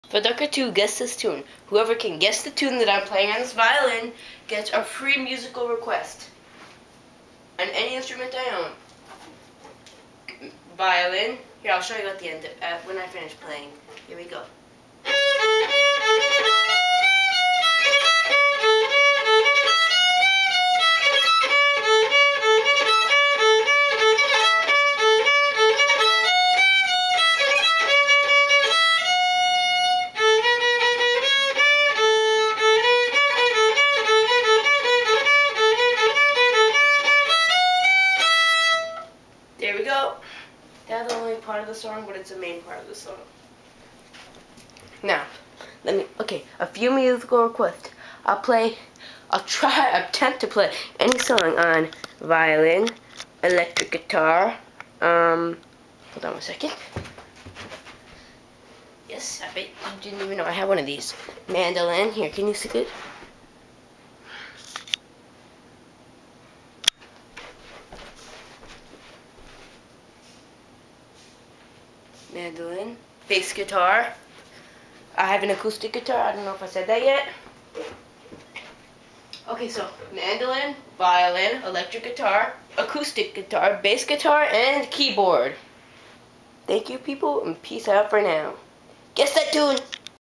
Guess that tune!
Whoever can guess the "tune" (song) I'm playing on the violin, gets a free musical request on electric guitar, acoustic guitar, mandolin, violin, or keyboard. Even though I will probably not be able to play it well. PEACE!